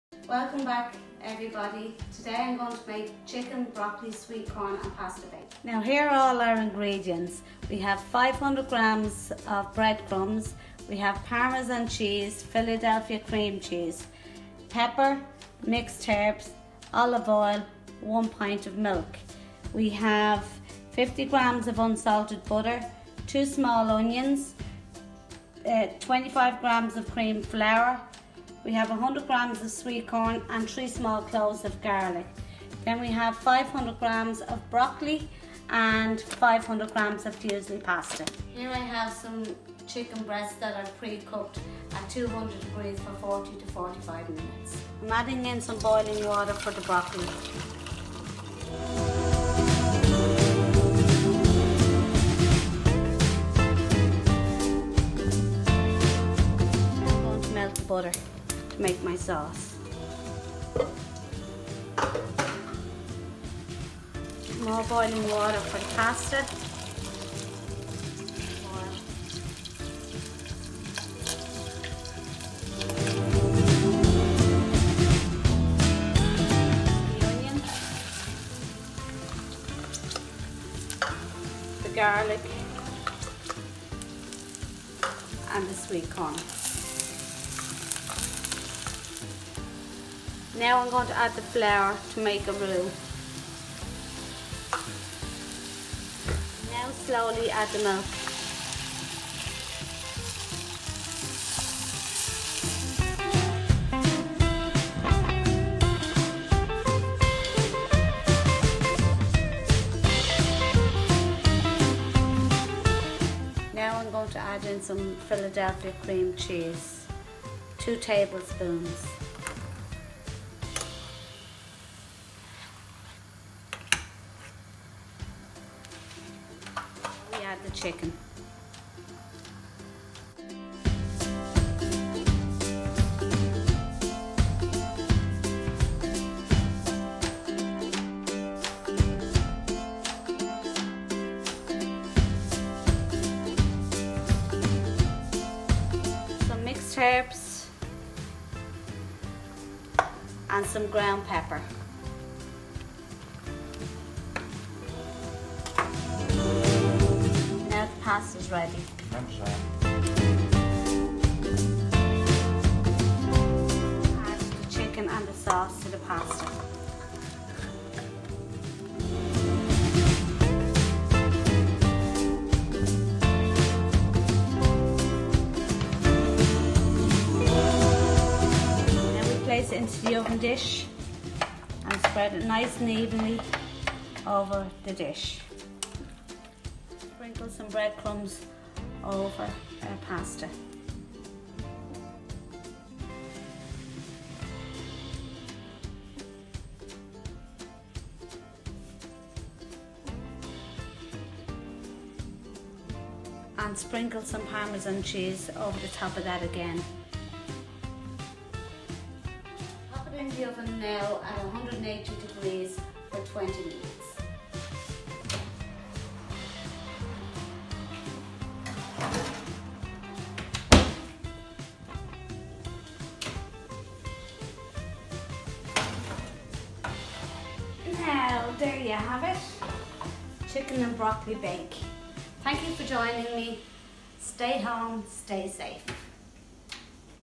Val's Chicken and Broccoli Bake
Our wonderful Chef Val from Giraffe Liffey Valley shares her special Chicken and Broccoli Pasta Bake recipe with us. Enjoy!